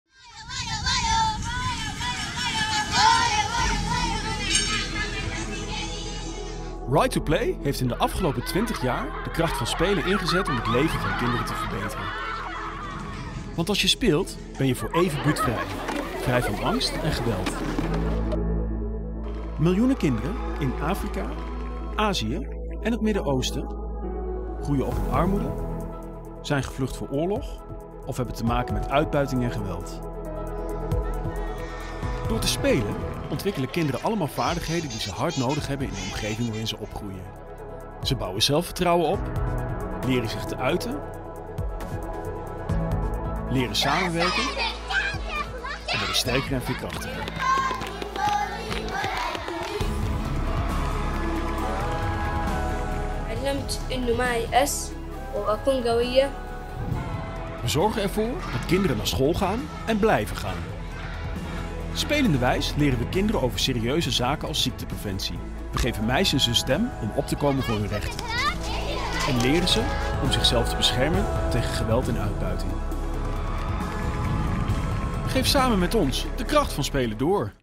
Right To Play - Spelen Met Impact
Miljoenen kinderen in Afrika, Azië en het Midden-Oosten groeien op in armoede, of hebben te maken met uitbuiting en geweld. Wij verbeteren het leven van deze kinderen met de kracht van spelen. Dat doen we al 20 jaar. Ontdek meer op